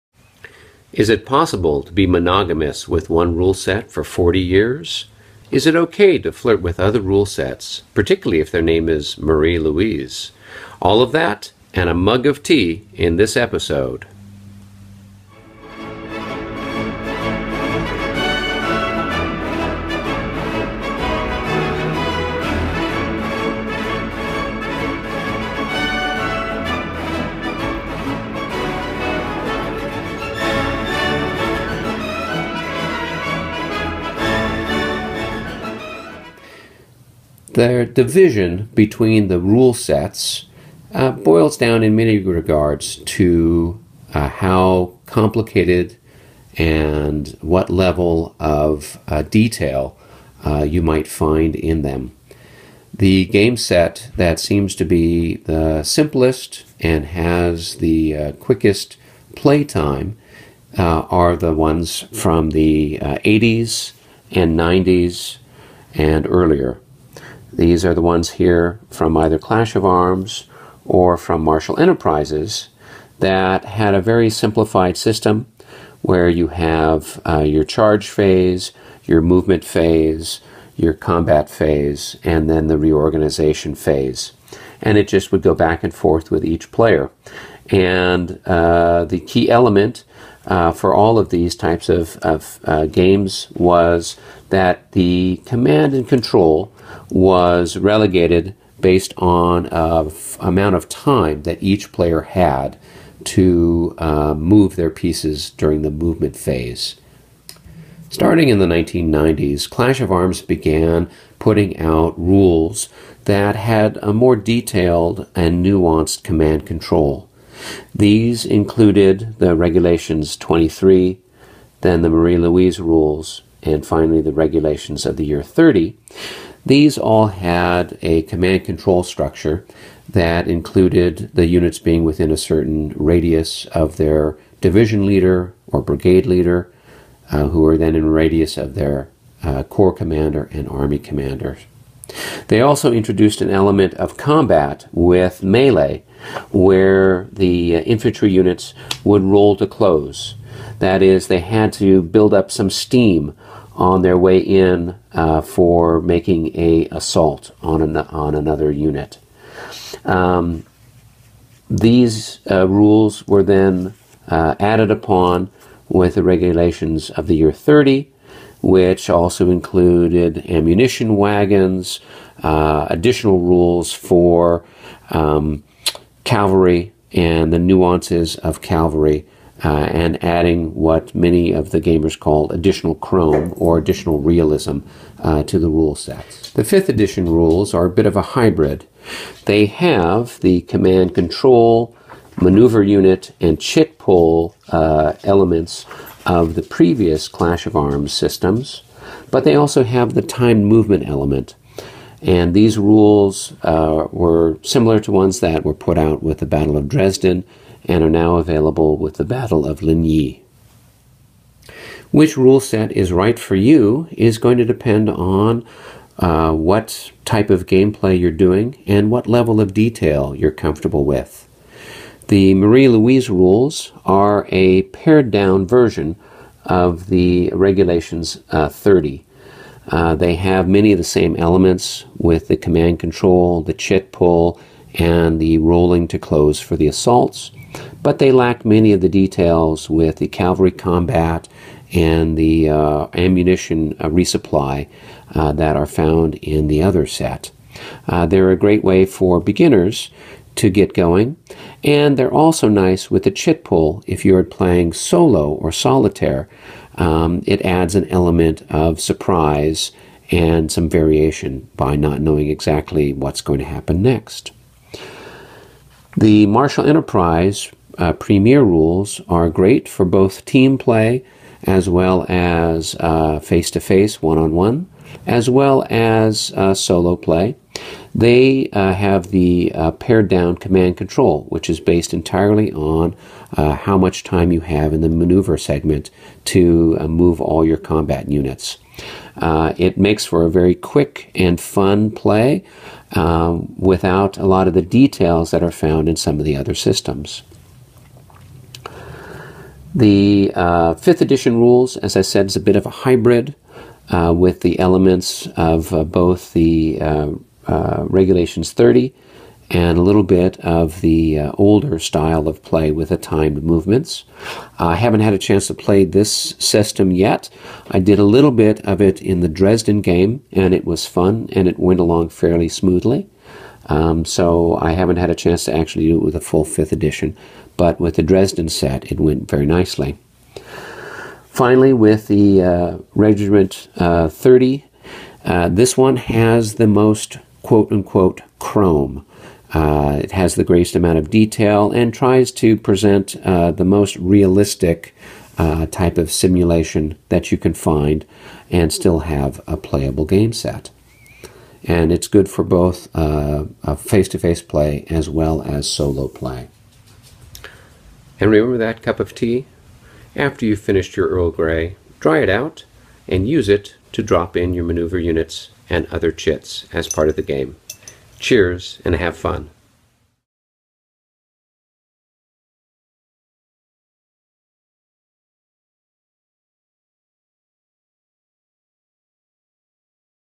La Bataille Rules Sets
A quick overview of the many rules sets for the Napoleonic game series.

The Premier rules and 3rd Edition Rules are the easiest to learn and play.  They have a simple alternating turn structure and easy sequence of play.  No command control and timed movment allows for a faster playing game.

The Marie-Louise rules are a good for solo games.  The chit pull mechanism creates variety.  These rules have a  more detailed command & control element that requires the players to keep their forces together in a way that mimics Napoleonic battle situations.  The ML rules are a simplified version of the Regs XXX.

The Regs XXX (30),  are a very well written set of rules.  Almost any situation that might arise in the game has been thought out and there is a handy rule to guide players.  Regs XXX includes several examples of play which can help newer players.

The 5th Edition are a hybrid of the older 3rd Edition and concepts from the Regs.  It has similar command control rules, chit pull and assault markers as Regs.  5th Edition does have timed movement and is ideal for team play.